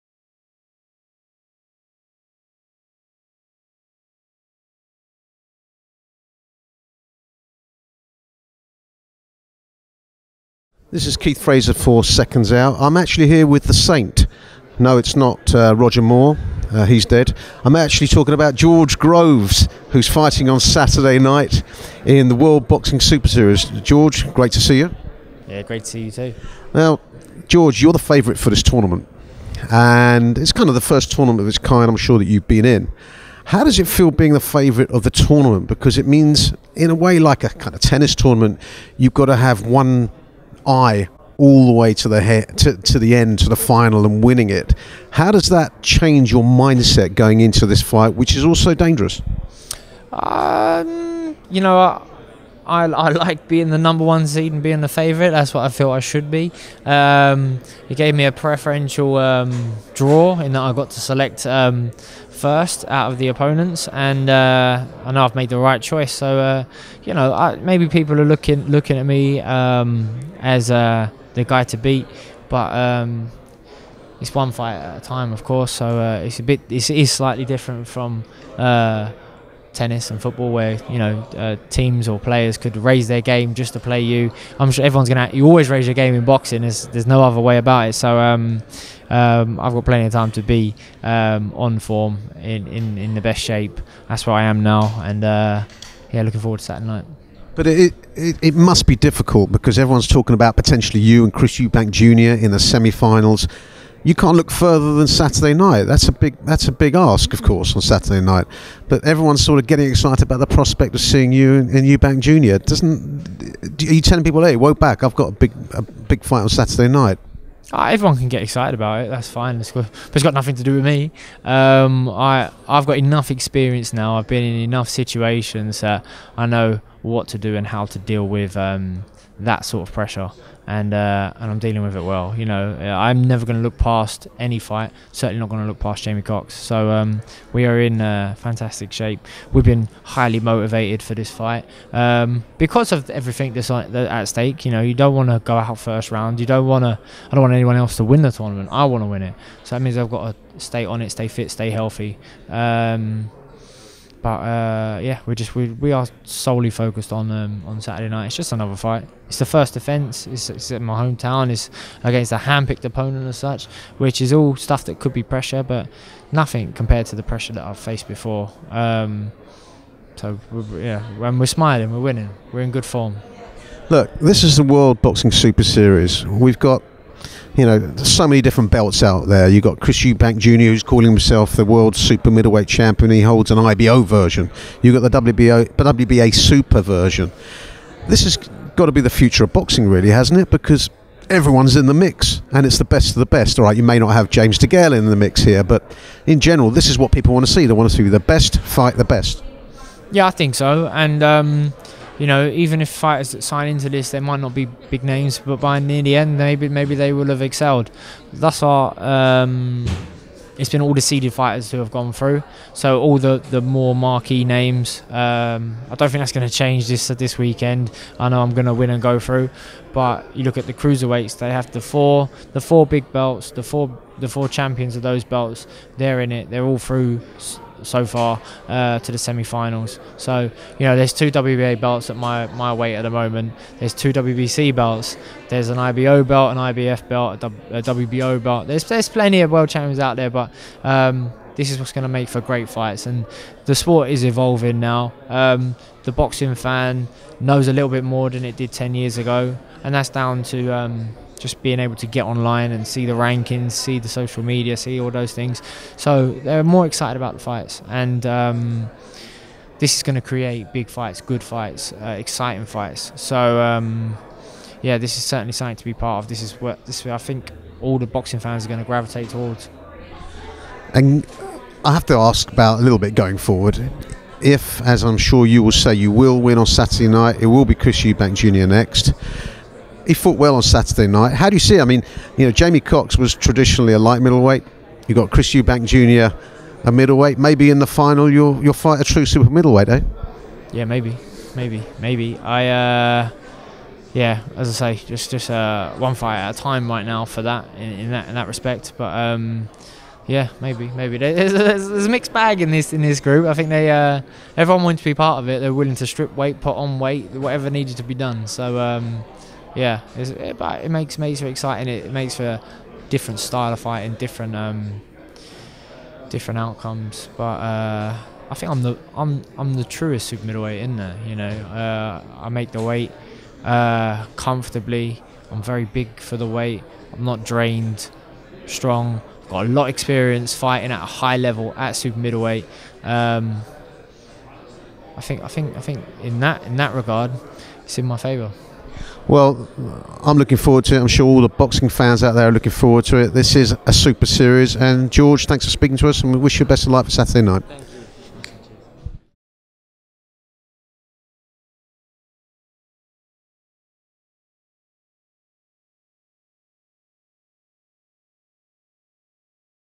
GEORGE GROVES Talks JAMIE COX Fight & Possible EUBANK JR Fight
GEORGES GROVE Talks JAMIE COX Fight & Possible EUBANK JR Fight | interviewed by Keith Fraser